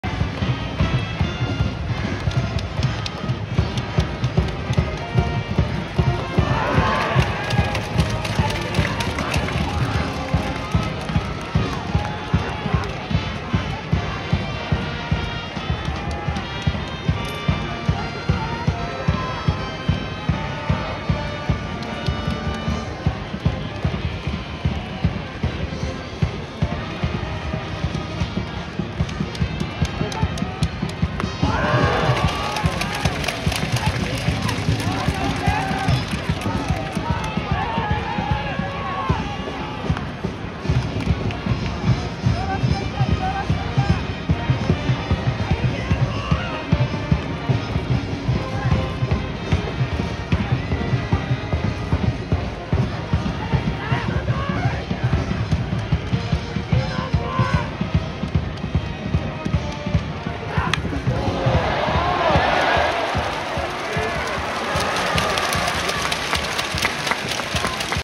早稲田大学、熊田任洋のすごい守備
2022/11/06
慶應義塾大学対早稲田大学
早稲田大学、熊田任洋のすごいセカンド守備。
廣瀬隆太の強いゴロをさばく。